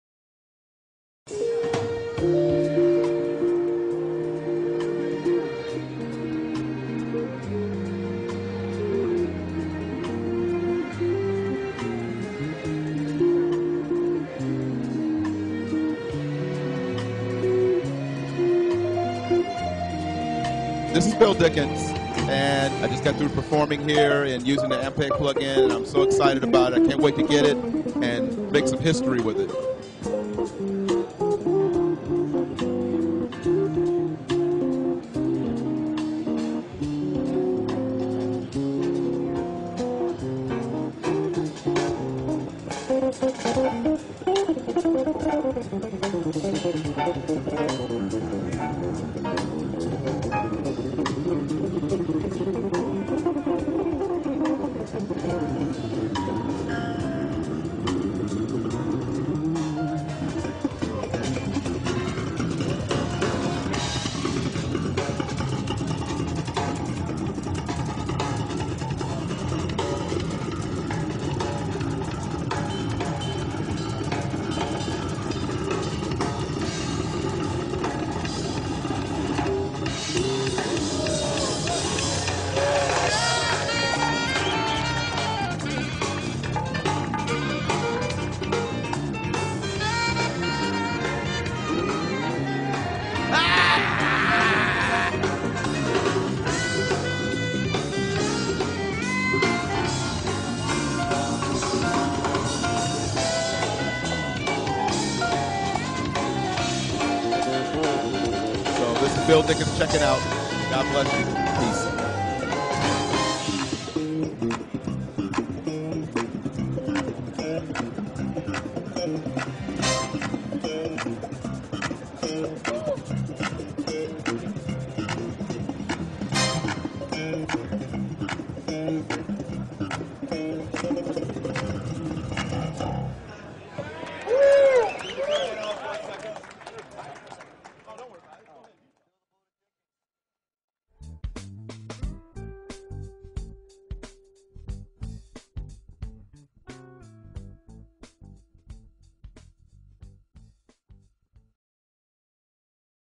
IK Multimedia "Ampeg SVX" | Product Demo
A legendary bass sound from the leaders in bass amplification and modeling technology. Ampeg has made history with its bass amp sound, and now sets the future standard with this premier software plug-in. Made in conjunction with IK Multimedia, the leader in software modeling technology, this utility sports Ampegs legendary signature tones.